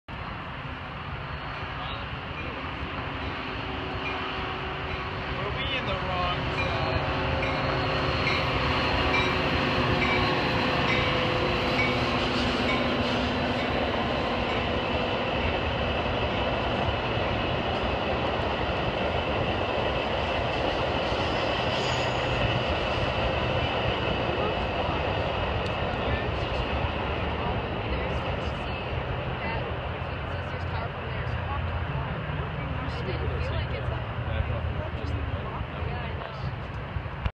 Metra F40PH-3 #165 Express Going Outbound Through Clybourn on the UP-N Line
(Filmed on 5/25/18).  This is my very first time filming trains pass by at this Clybourn station.  The nice thing about it is that it gives you access to both the Union Pacific Northwest Line & Union Pacific North Line.  In this video, you will see an F40PH-3 #165 express go outbound on the north line.